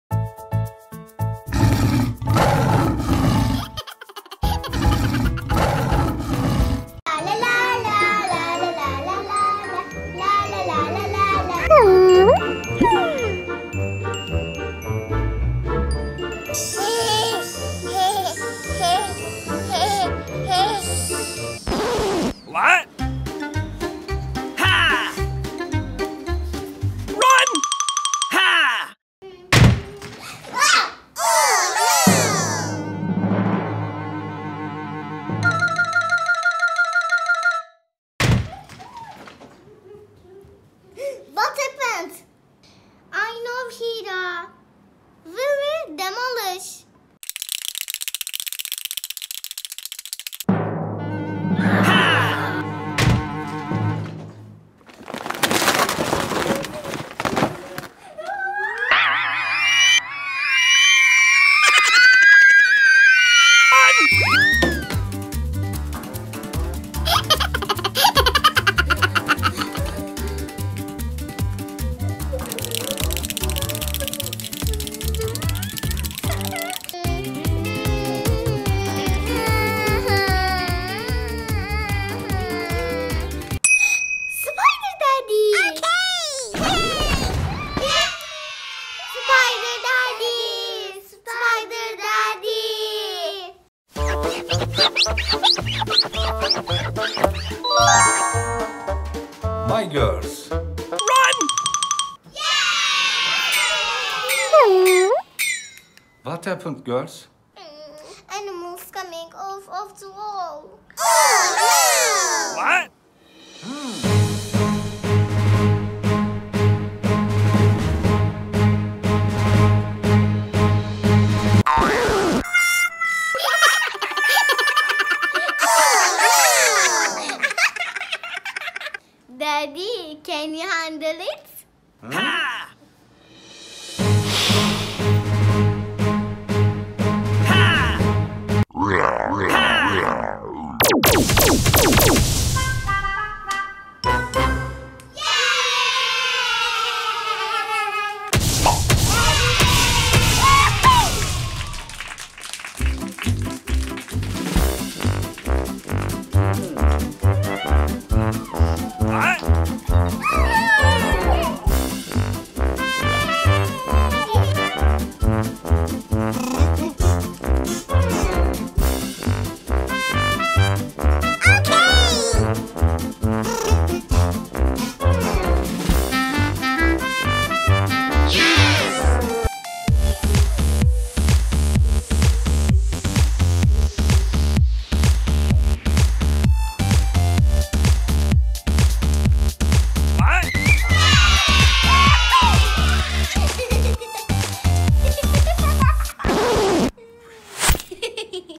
superhero and kids video
kids clean germs with their superhero